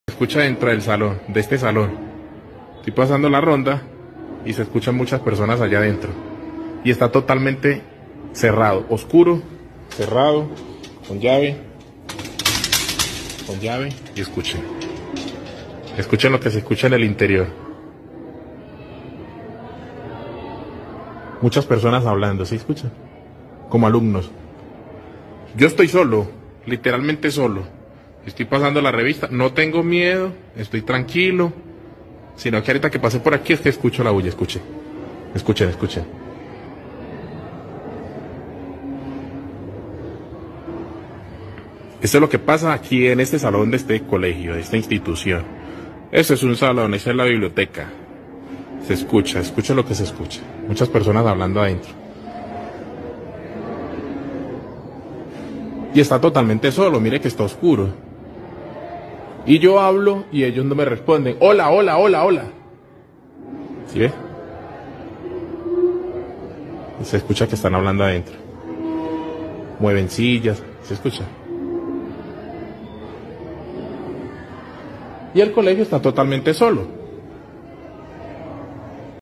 Medellín, salón de un colegio embrujado
Chequen por usted mismos esta noticia que está dando Vueltas en el
Mundo, esto
Sucedió en Colombia en un colegio en Medellín, donde se escuchan voces y ruidos de mesas última sillas totalmente solos.